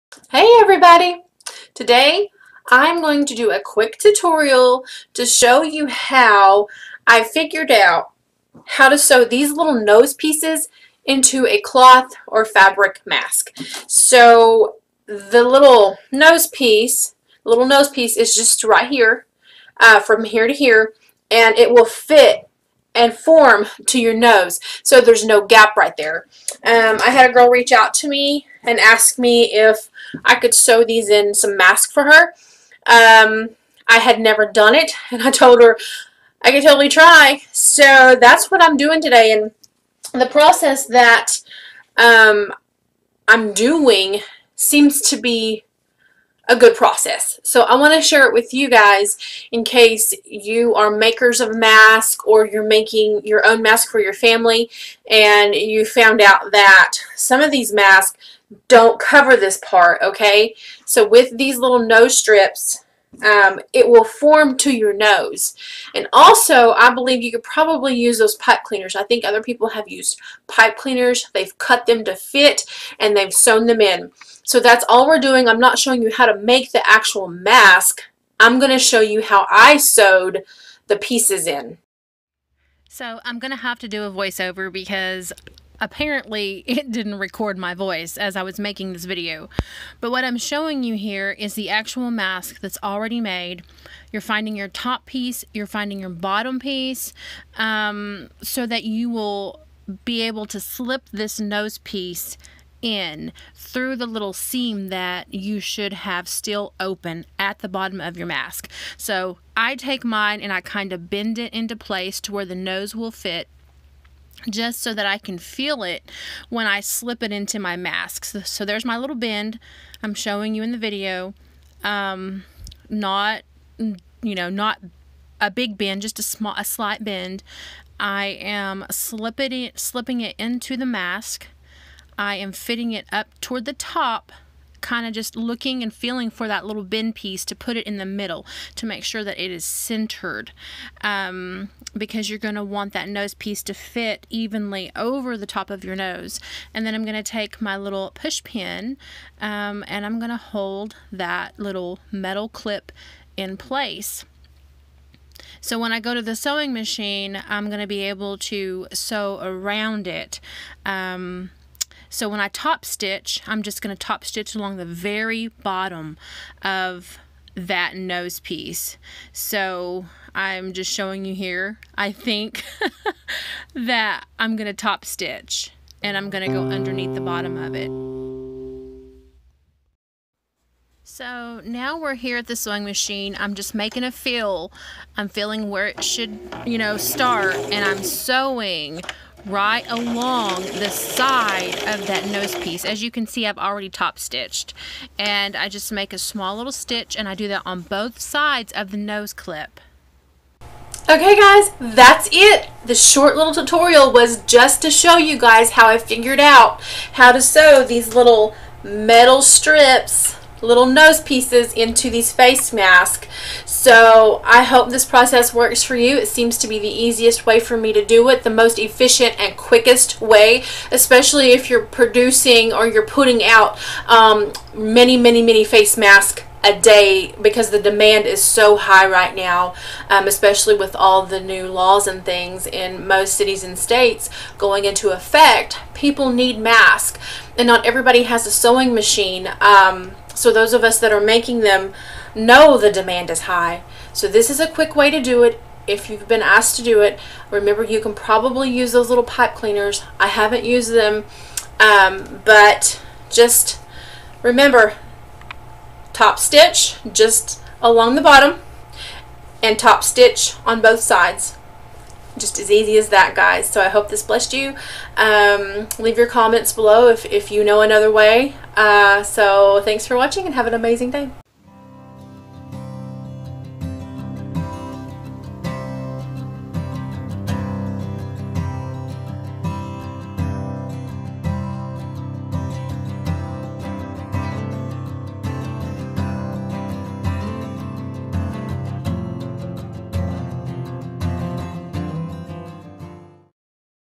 How To Sew A Nose Piece In A Face Mask
A sewing tutorial on how to sew in a metal nose piece into a face mask.

You are invited to our church, Christian Worship Center